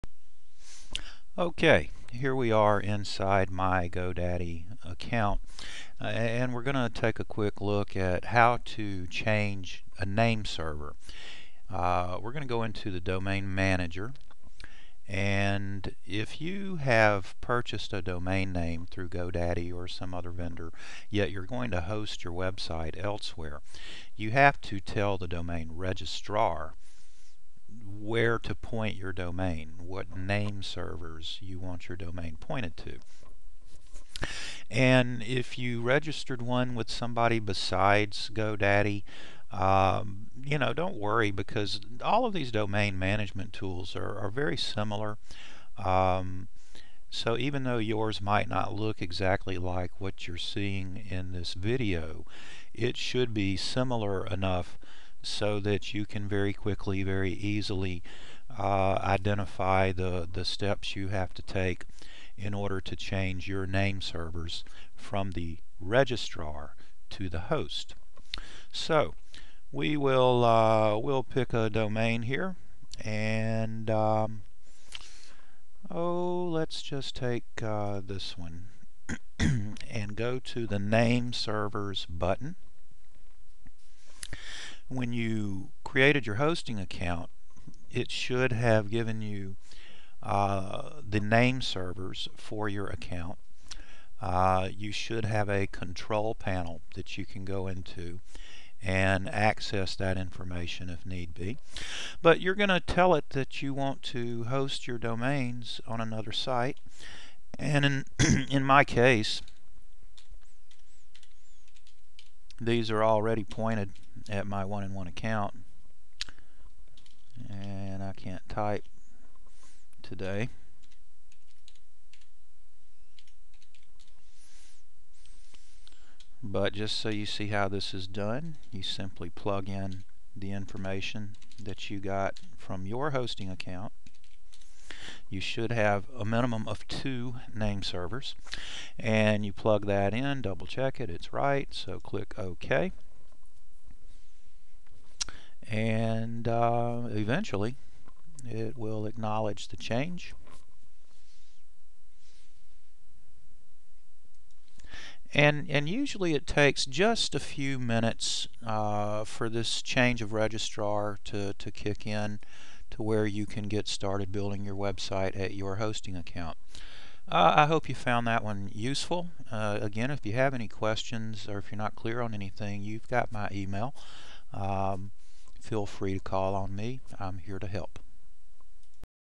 How to change the name servers from your domain registrar to your hosting account.
If you are hosting your web site someplace different from where you registered your domain; you will have to change your names servers in order to "point" your domain name to your hosting account.